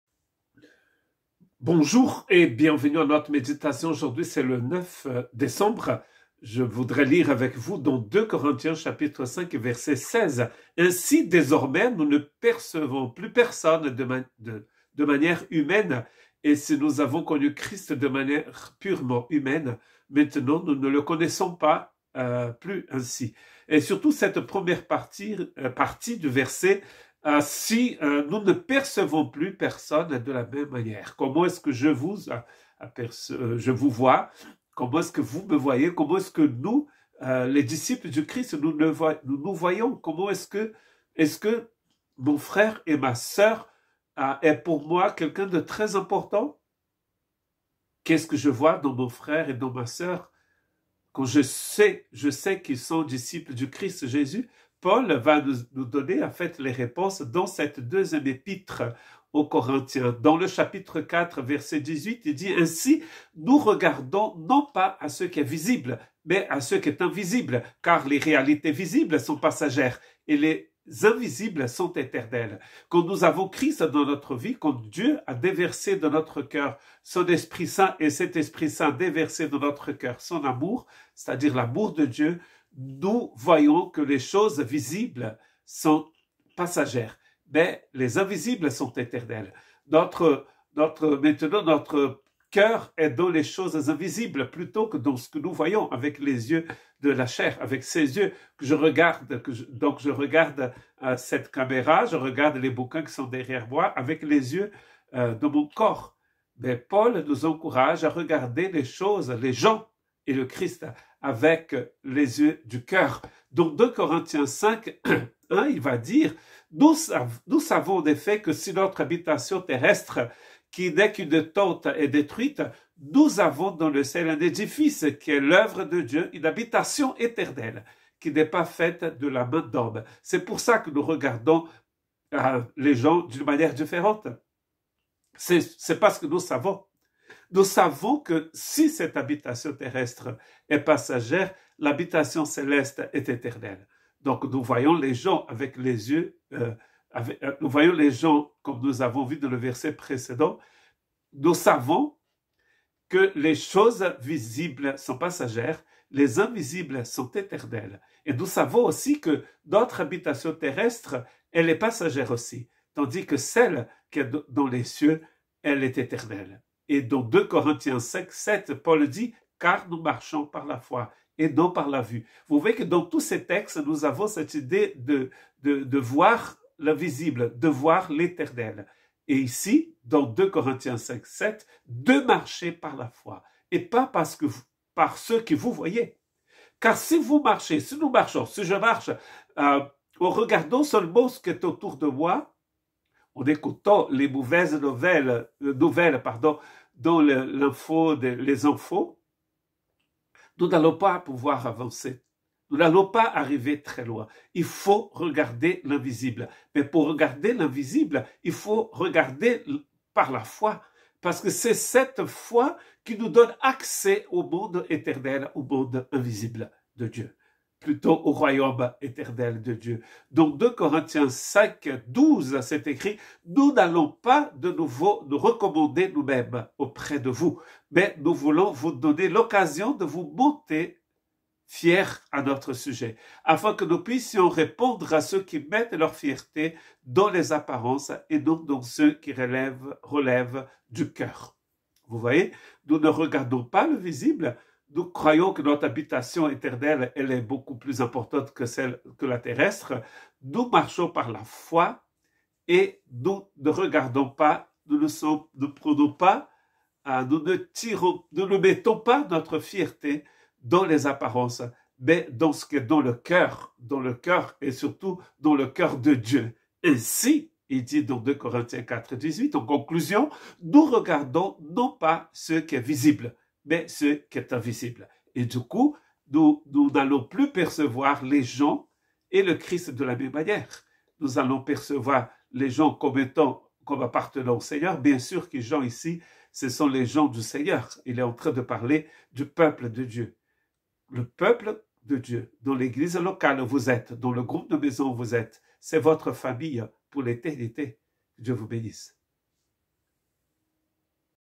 🇲🇫 09 décembre 2023. Nous ne percevons plus personne de manière humaine! Junior Damaceno
Ainsi, désormais, nous ne percevons plus personne de manière humaine; et si nous avons connu Christ de manière purement humaine, maintenant nous ne le connaissons plus ainsi.
2 Corinthiens 5:16